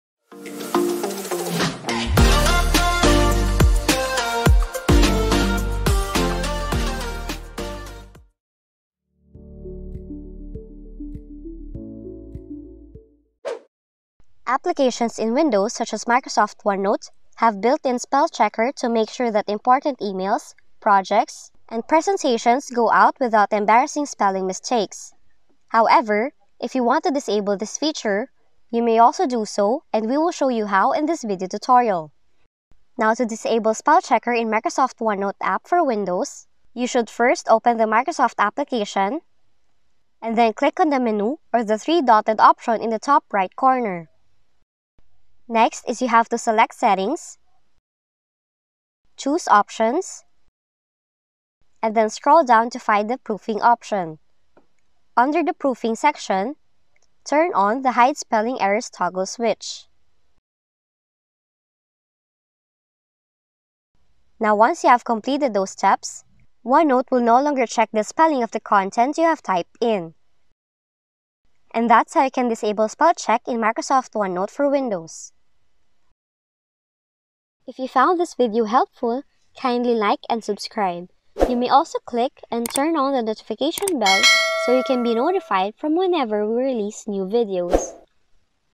OneNote - Easily DISABLE (or Enable) Spell Check!
Would you like to disable the spell check feature in OneNote?  Sometimes this feature can drive you mad if you're using unusual words that really aren't misspelled, or for any other reason you don't want to keep seeing that annoying squiggly red underline.  Watch this video to learn exactly how to disable spell check in OneNote.